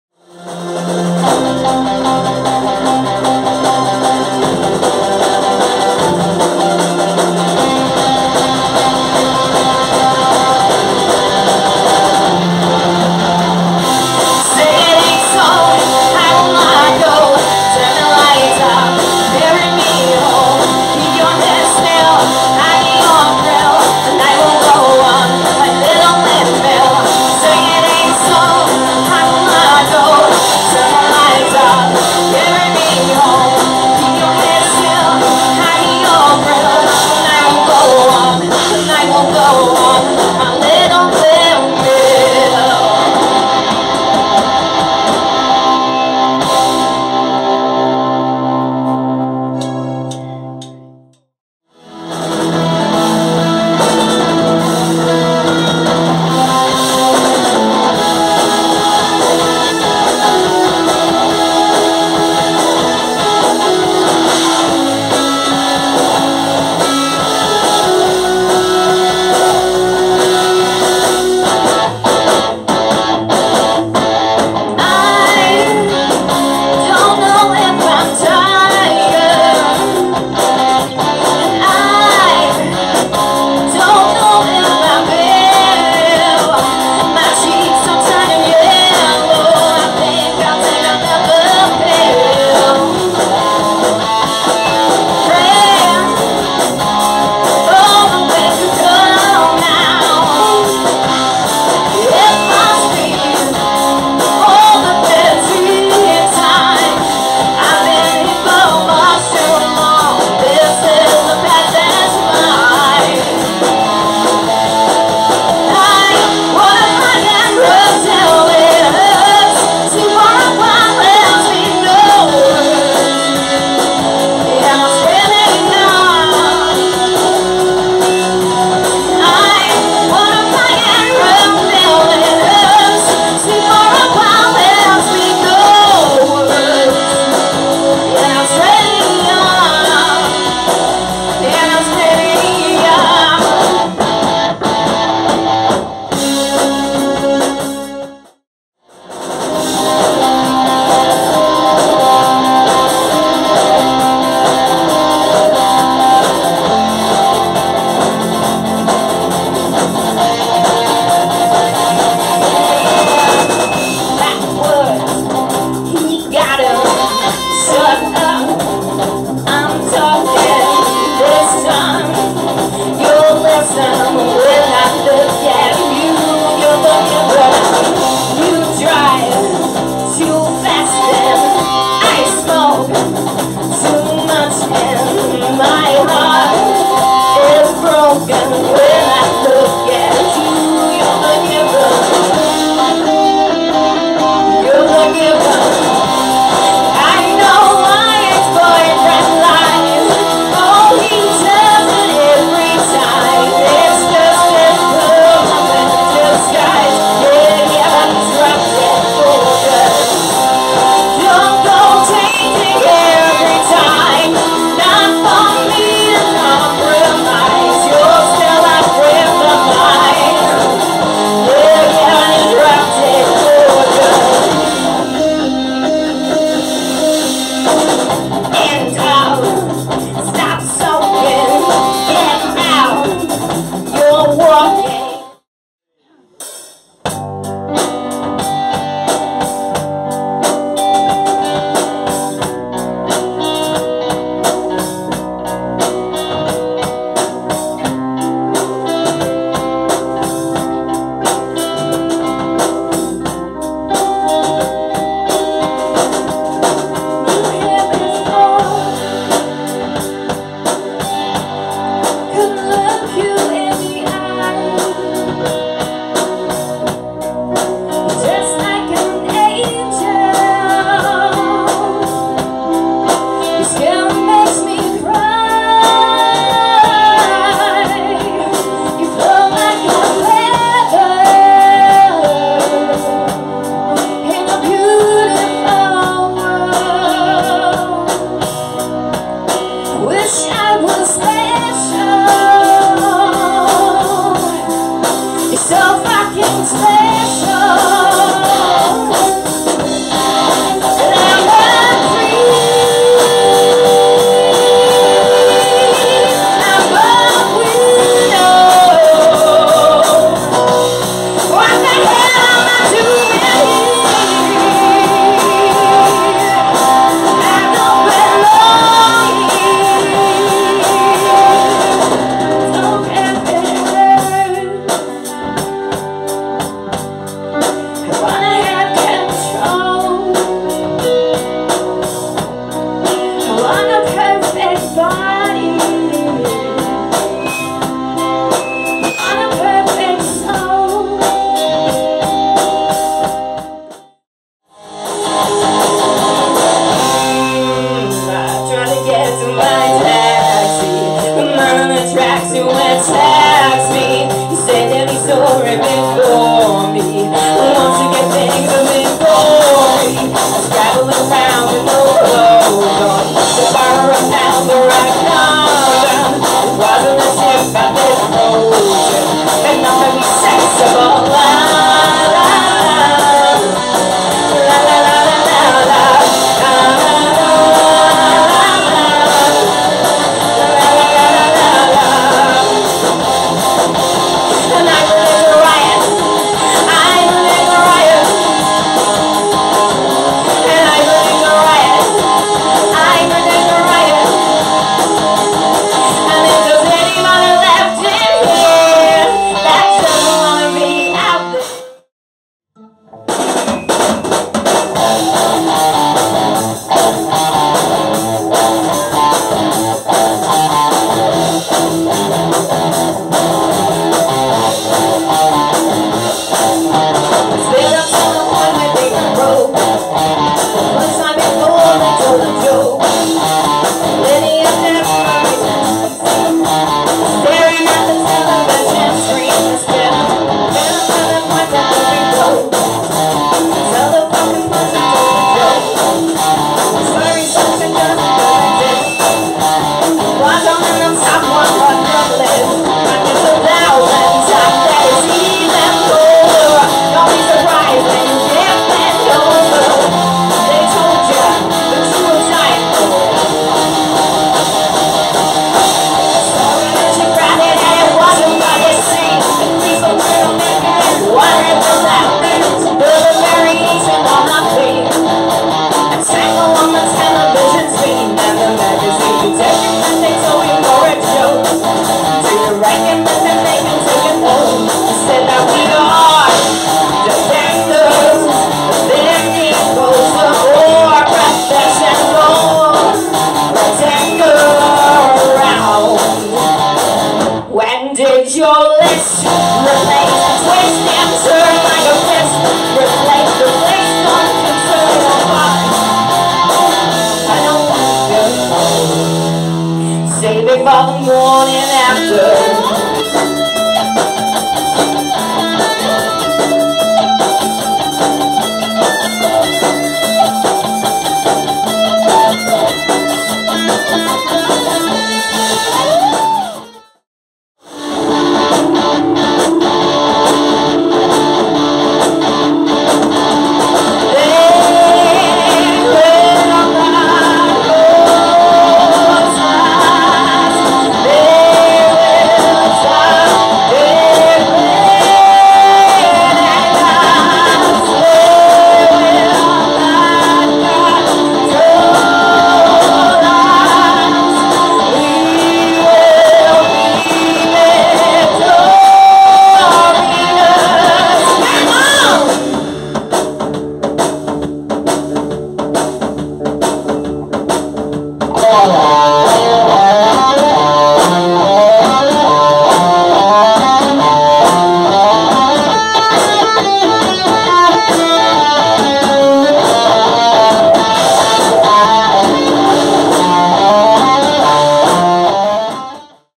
Total Recovery - Practice Session 09-Sept-2024
Some snippets from the practice session by Total Recovery at Als session room. Thirsty work, but He looks after us with liquid refreshment. hehe.

Second time for TR with Kat on vocals, throwing all our stuff at her with little warning, as is normal. Always a laugh.

Total Recovery are a local rock band, Al on guitar / vocals, Vickie on keyboards, (hidden by the music stand).  Daz, the powerhouse, on drums, and me in my alter-ego as bassist.